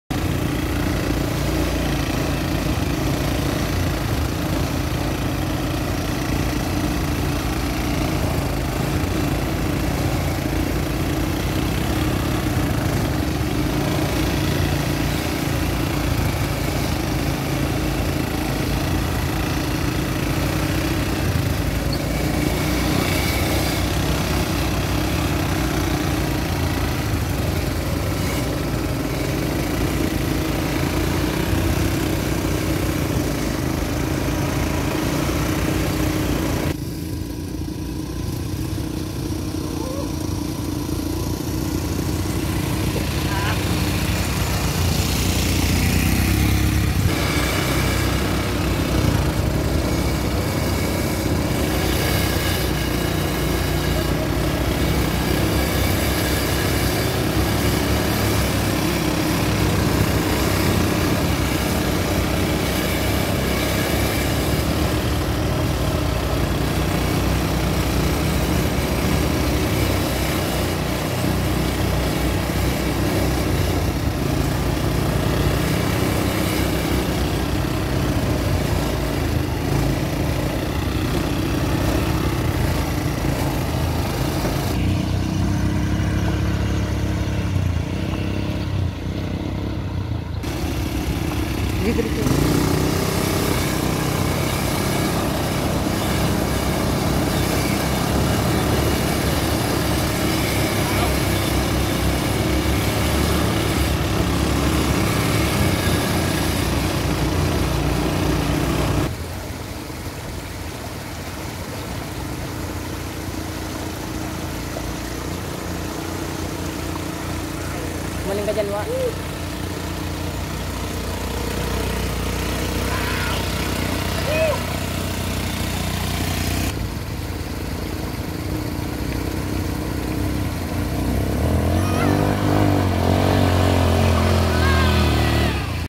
ATV Adventure Ride
Explore the beauty of nature at the vicinity of Mayon Volcano